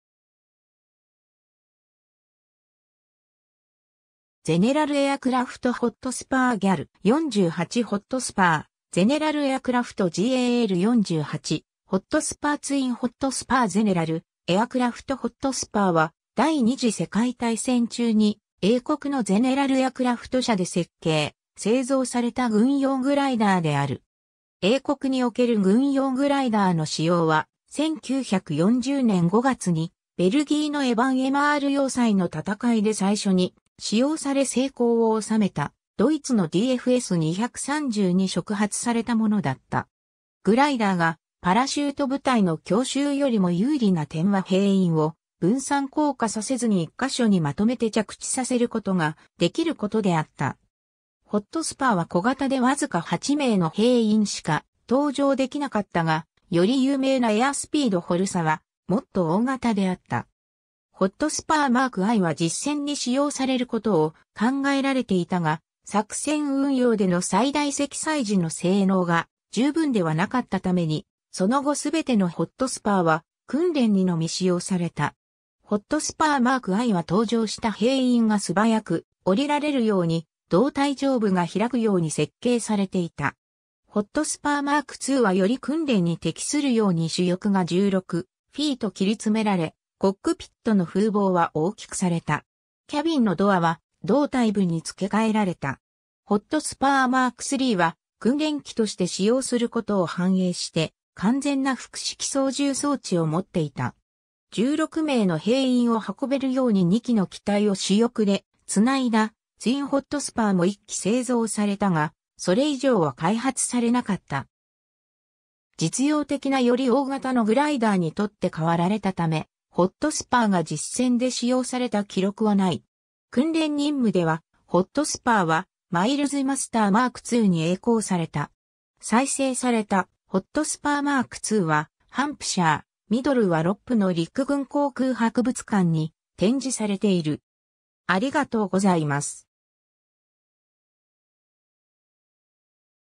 ゼネラル・エアクラフト ホットスパー
ゼネラル・エアクラフト ホットスパー GAL.48 ホットスパー General Aircraft G.A.L. 48 Hotspur Twin Hotspur ゼネラル・エアクラフト ホットスパー（General Aircraft Hotspur）は、第二次世界大戦中に英国のゼネラル・エアクラフト社（General Aircraft Limited）で設計、製造された軍用グライダーである。
 英国における軍用グライダーの使用は、1940年5月にベルギーのエバン・エマール要塞の戦いで最初に使用され成功を収めたドイツのDFS 230に触発されたものだった。
グライダーがパラシュート部隊の強襲よりも有利な点は兵員を分散降下させずに1箇所にまとめて着地させることができることであった。
 ホットスパーは小型で僅か8名の兵員しか搭乗できなかったが、より有名なエアスピード ホルサはもっと大型であった。
ホットスパーMk Iは実戦に使用されることを考えられていたが、作戦運用での最大積載時の性能が十分ではなかったためにその後全てのホットスパーは訓練にのみ使用された。
 ホットスパー Mk Iは搭乗した兵員が素早く降りられるように胴体上部が開くように設計されていた。
ホットスパーMk IIはより訓練に適するように主翼が16 ft (4.88 m)切り詰められ、コックピットの風防は大きくされた。
キャビンのドアは胴体部に付け替えられた。
ホットスパー Mk IIIは訓練機として使用することを反映して完全な複式操縦装置を持っていた。
 16名の兵員を運べるように2機の機体を主翼で繋いだツイン ホットスパーも1機製造されたが、それ以上は開発されなかった。
 実用的なより大型のグライダーに取って代わられたためホットスパーが実戦で使用された記録はない。
 訓練任務ではホットスパーはマイルズ マスター Mk IIに曳航された。
 再生されたホットスパーMk IIはハンプシャー、ミドル・ワロップ（Middle Wallop）の陸軍航空博物館（Army Museum of Flying）に展示されている。
 （ホットスパー Mk 2）